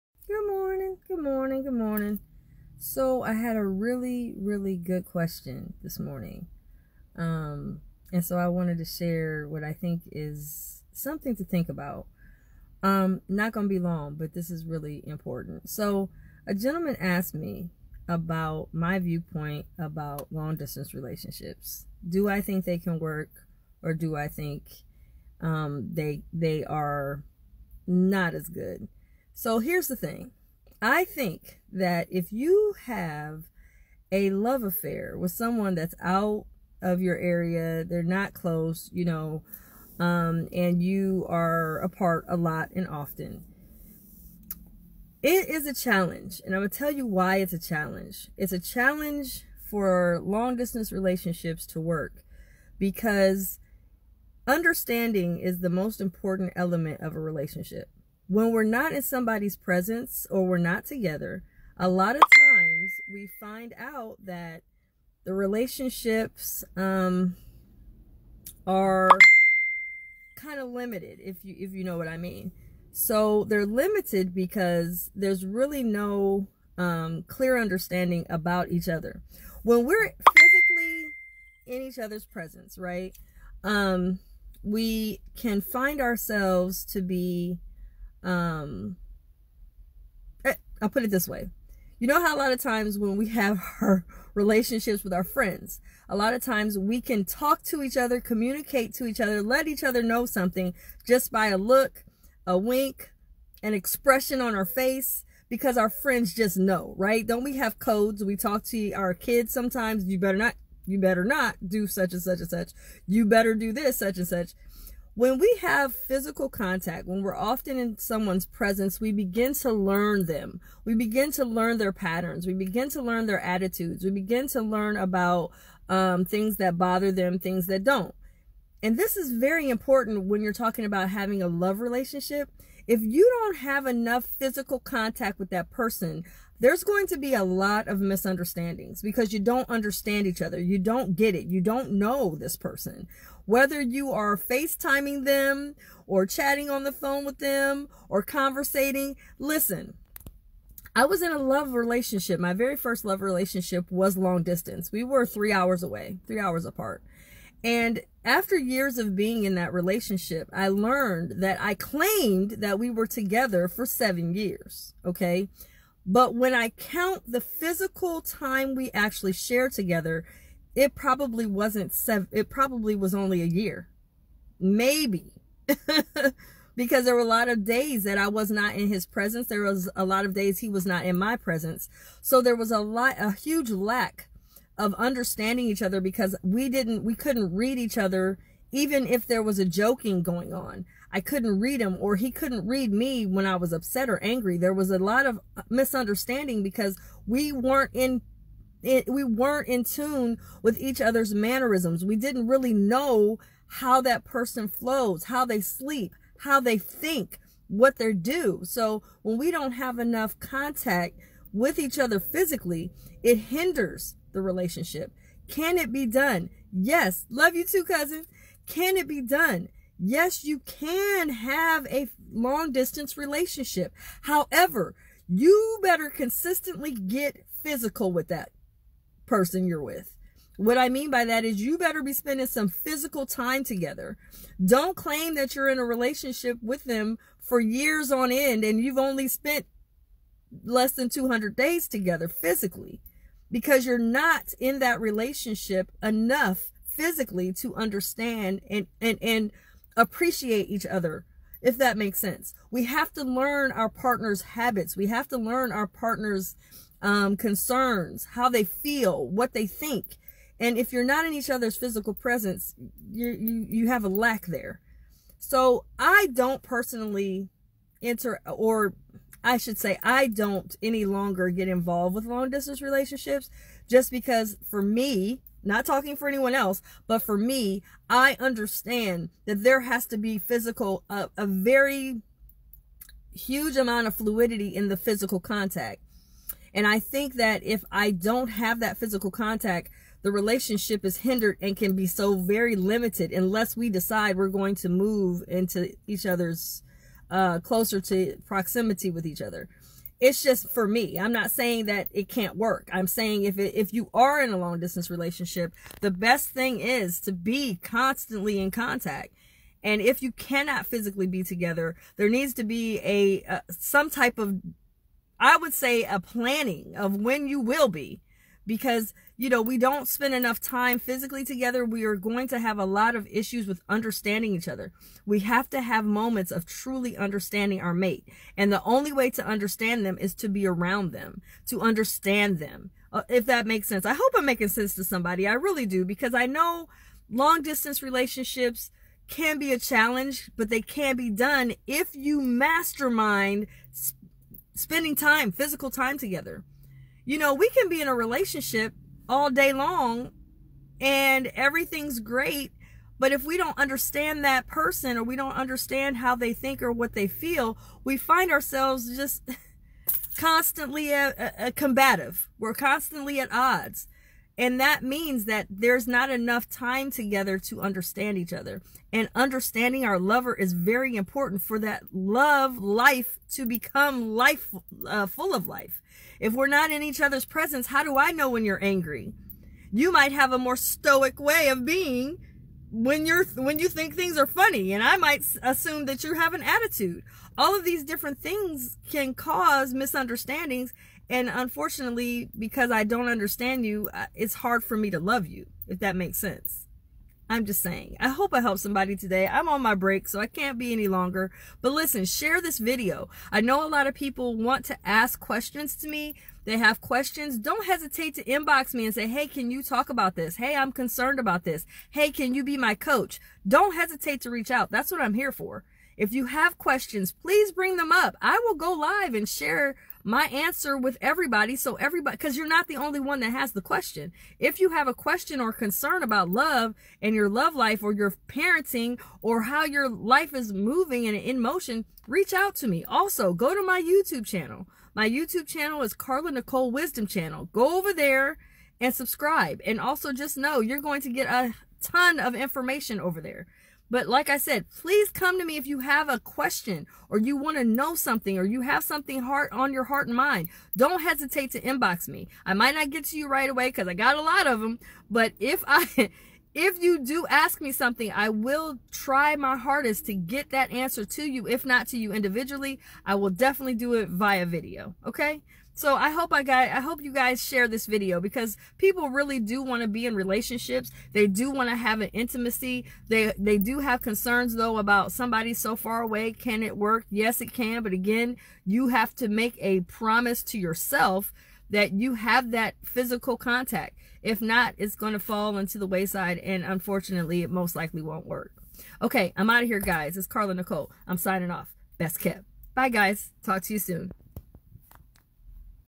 LONG DISTANT LOVE AFFAIR CHALLENGES (Self-Reflection, Love Affairs, Relationships, Intimacy)
I had a personal message from a viewer asking my opinion about long distant relationships. He wanted to know if I think they can work and what are the challenges. Take a listen.